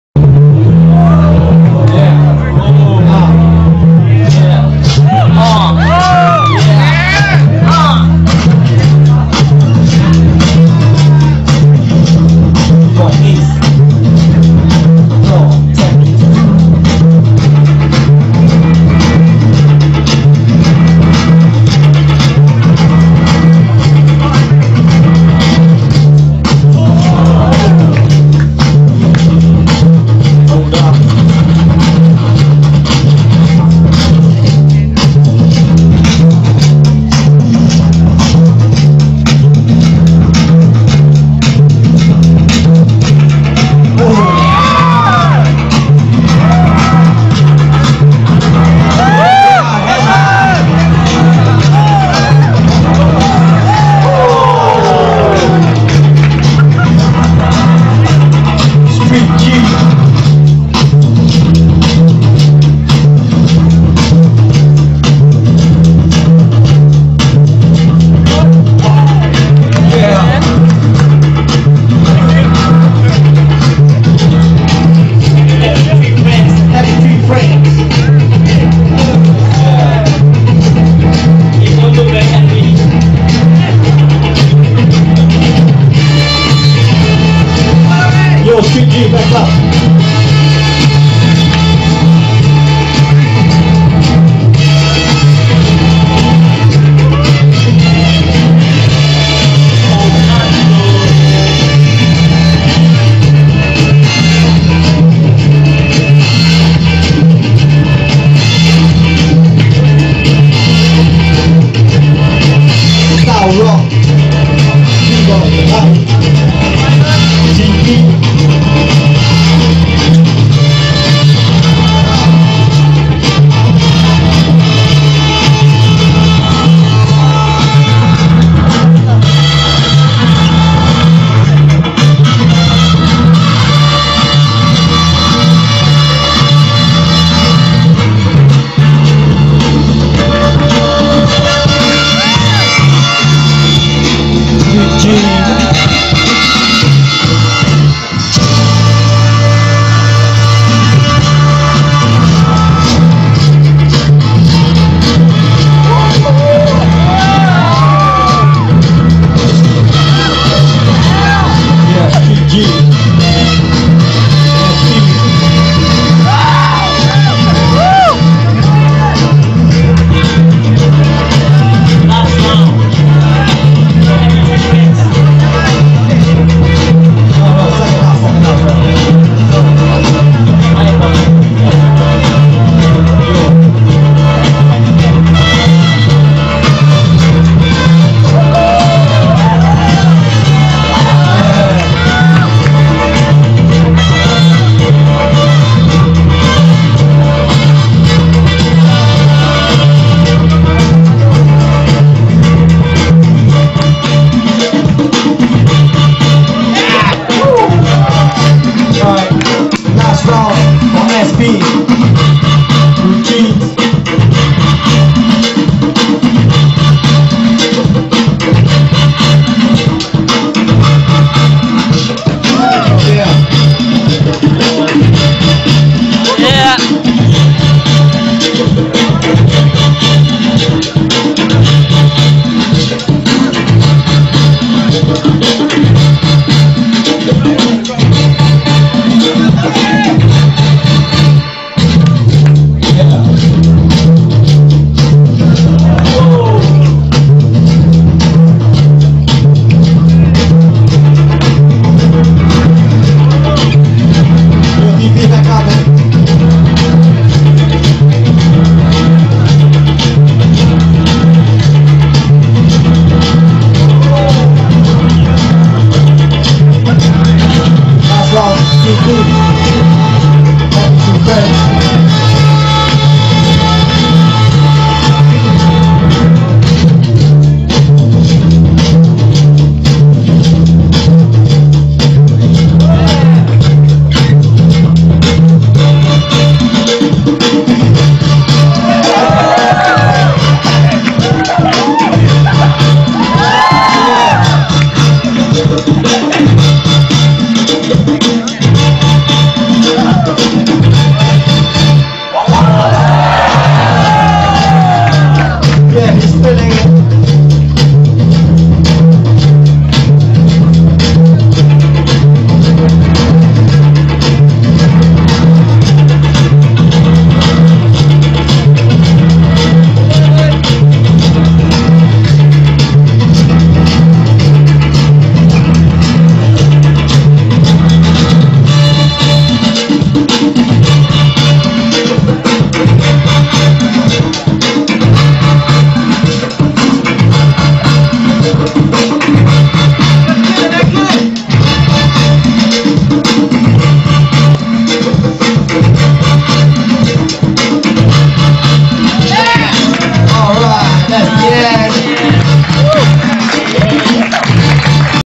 FuYo Inter School 2009: Happy3Friends(TP) vs Street G(SP)
This is the Quater-final battle in FUYO INTER SCH 2009.

Happy3Friends team sheet:
BBOY Erin (Cypher Crazed Crew, Bakau Rockers, Juicy Juice)
BBOY Sherman (Bakau Rockers)
BBOY Style-Roc (Cypher Crazed Crew & Juicy Juice)

Result: Draw, Happy3Friends won eventually after xtra rounds.